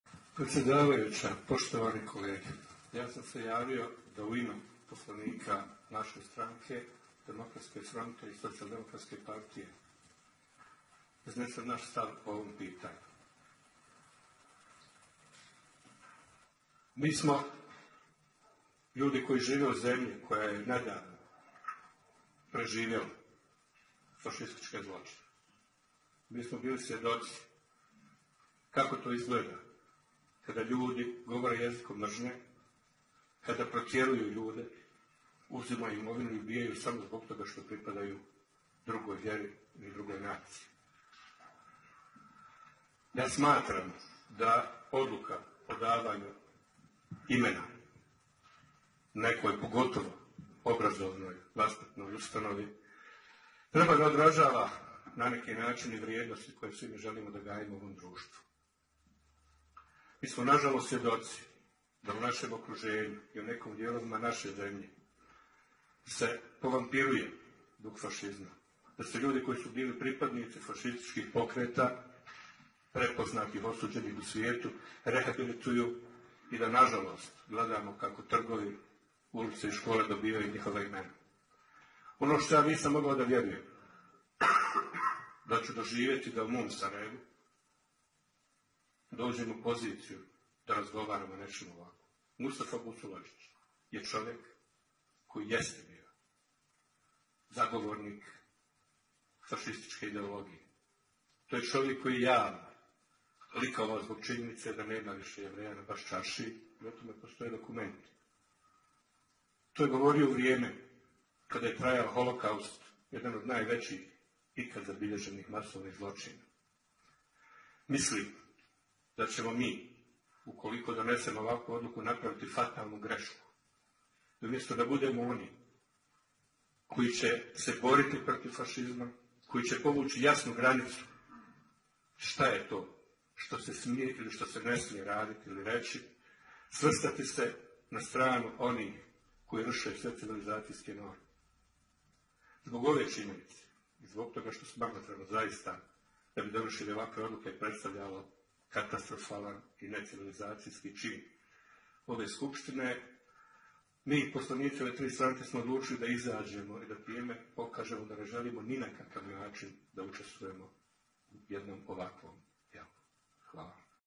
Šućur: Mustafa Busuladžić je bio zagovornik fašističke ideologije
Radi se o čovjeku koji je likovao zbog toga što više nema Jevreja na Baščaršiji